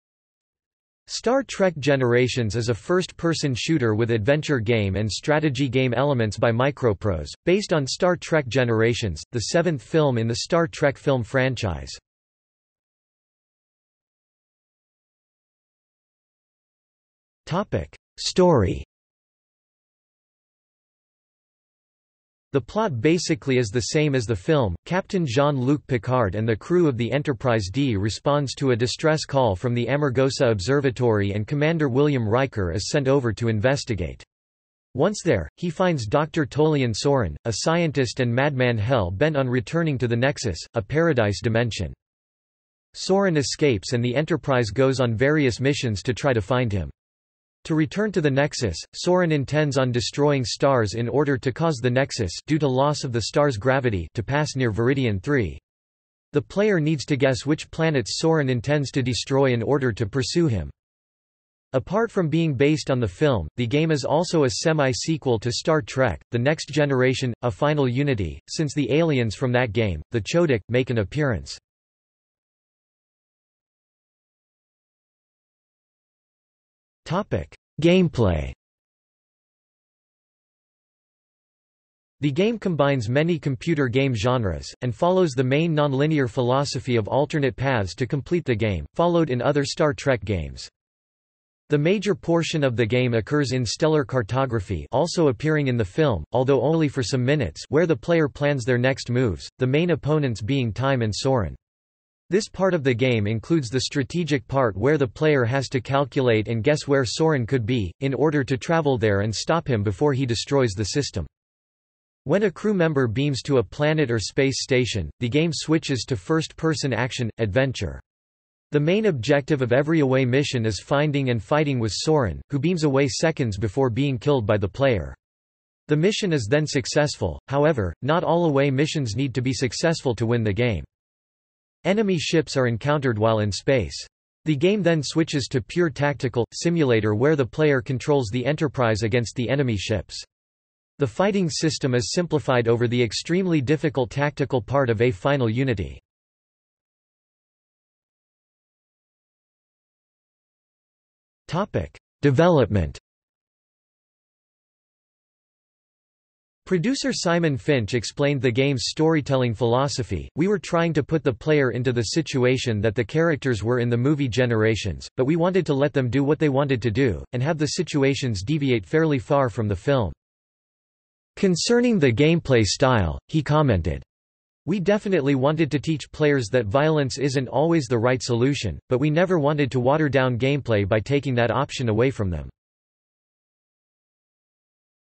Star Trek Generations (video game) | Wikipedia audio article
00:00:13 1 Story
00:01:13 2 Gameplay
00:02:27 3 Development

- Socrates

SUMMARY
Star Trek Generations is a first person shooter with adventure game and strategy game elements by MicroProse, based on Star Trek Generations, the seventh film in the Star Trek film franchise.